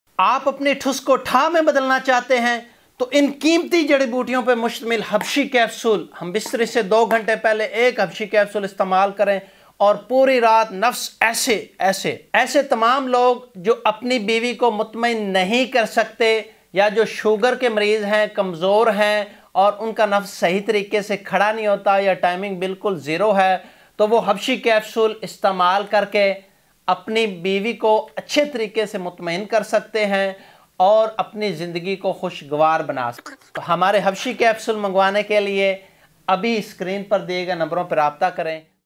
Mardana Taqat Ma izzafa
Aaj ke is video mein hum baat karenge mardana taqat aur sehat barhane ke kuch asaan aur effective tareeqon ke bare mein. Agar aap apni energy levels ko boost karna chahte hain, apne overall health ko behtar banana chahte hain, ya phir apni fitness ko improve karna chahte hain, to yeh video aapke liye hai!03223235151